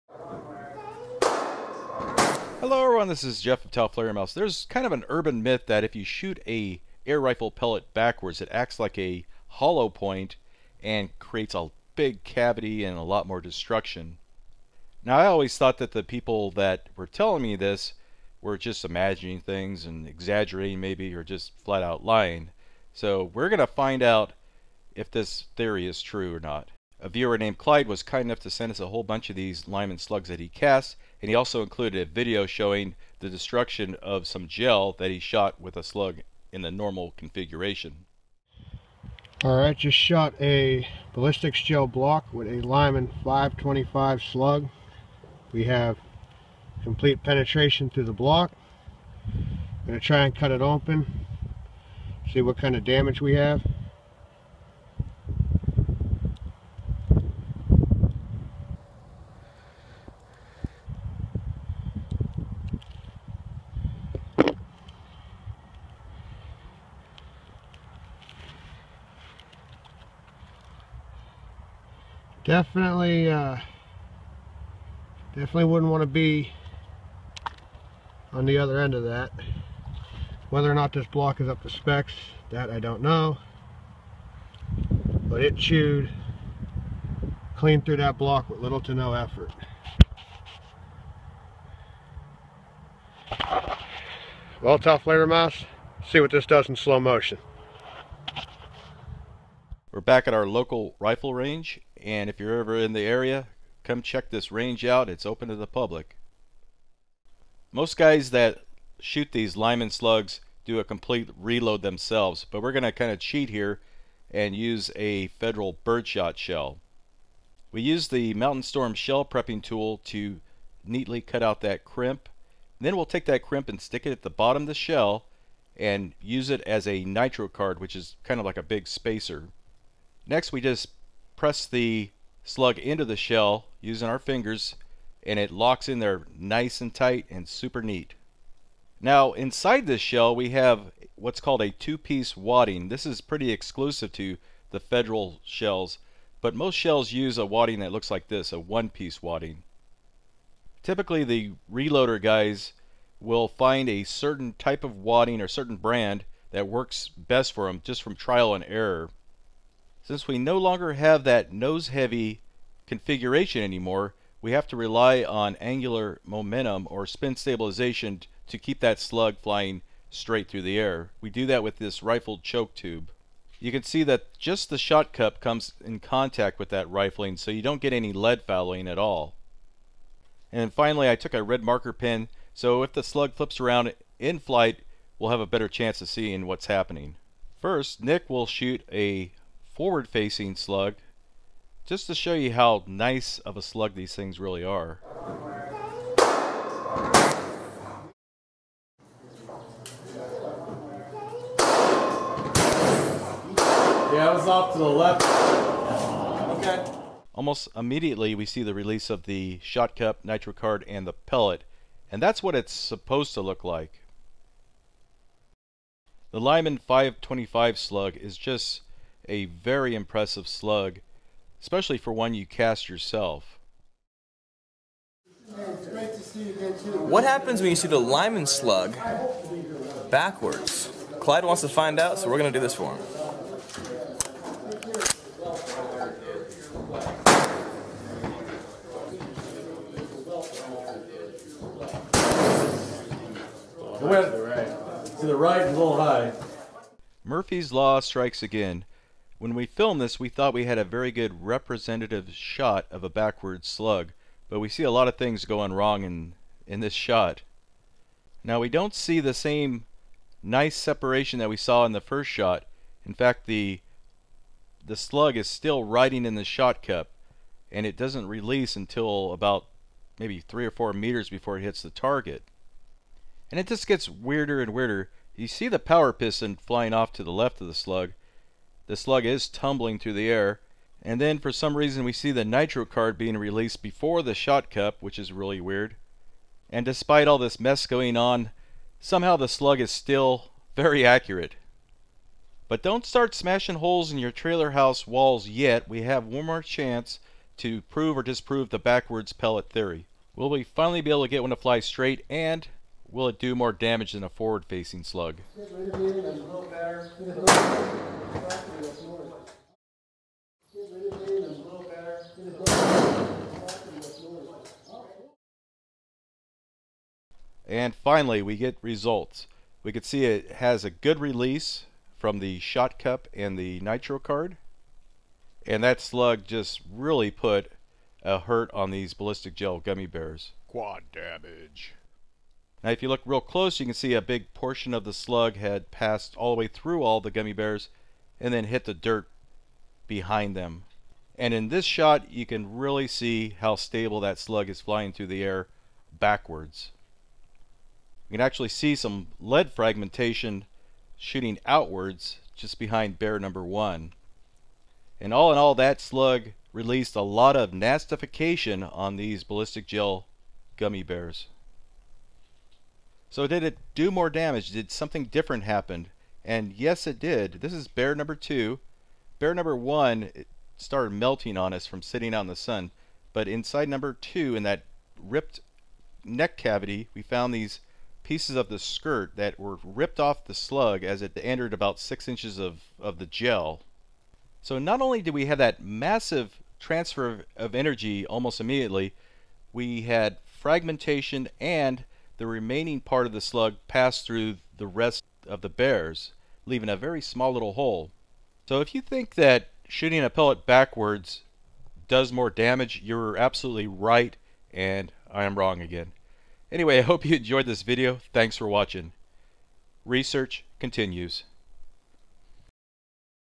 Does Shooting Slugs BACKWARDS Make them DEADLIER?
Many people who have had an air rifle have gotten bored or curious and tried shooting the pellets backwards.   People have sworn the backwards pellets are much more powerful and do more destruction.   Is it all in their minds?
 Clyde sent us a bunch of Lyman 525 slugs that we have experimented with.   We shot some with big holes drilled through the centers,  shot some with carpet tacks since there was a myth that doing so would make them "armor-piercing".
NOW we can try the myth of shooting pellets backward on a grand scale.